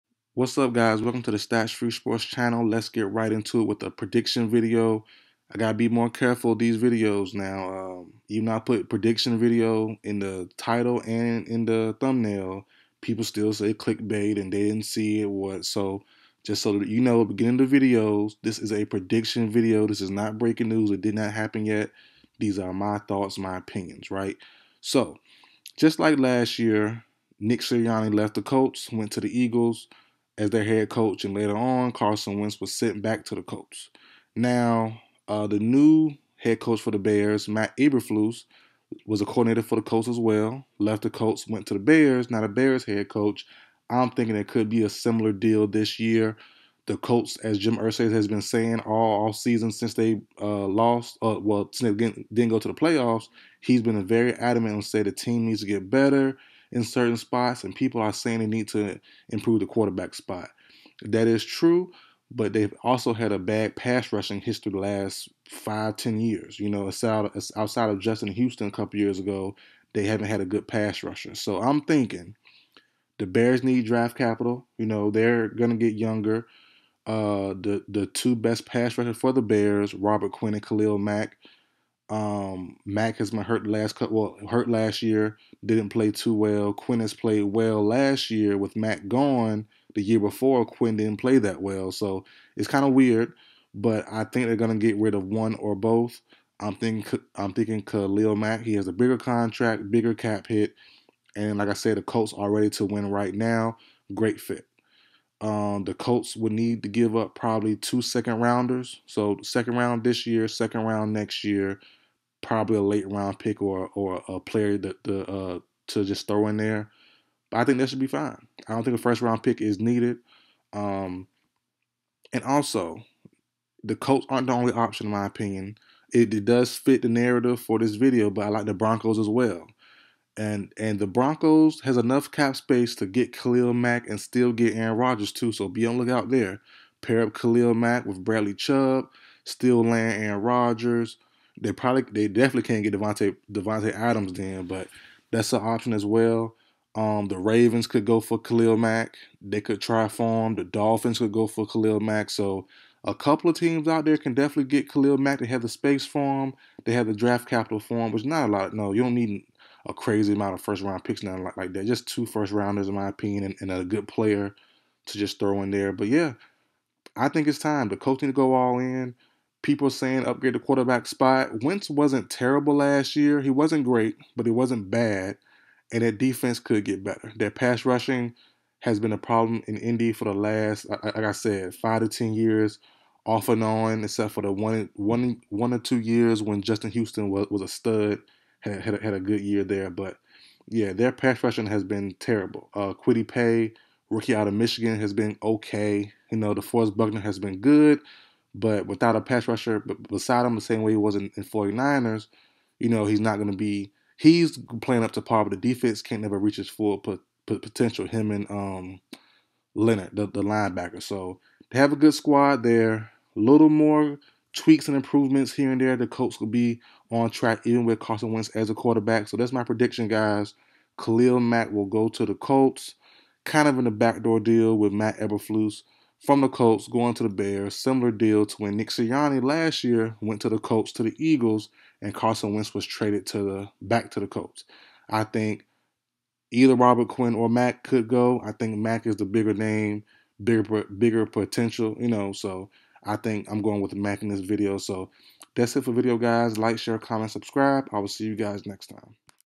Khalil Mack will be TRADED to the Colts | NFL Prediction
Could the Head Coach hire of Matt Eberflus be a sign that the Bears are trading Khalil Mack to the Colts or am I reaching?